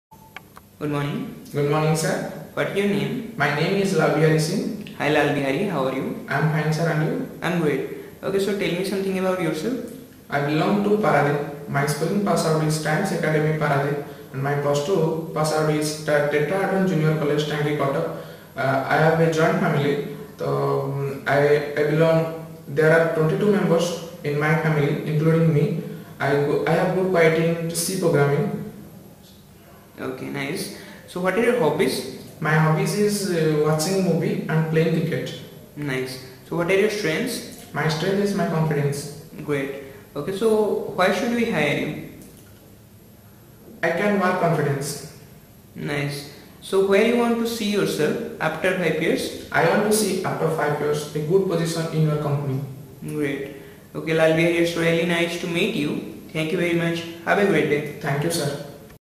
GIFT | E-RESUME | LAL BIHARI SINGH | CSE | 2021 BATCH
Gandhi Institute For Technology (GIFT), Bhubaneswar
Student Name: LAL BIHARI SINGH
Branch : CSE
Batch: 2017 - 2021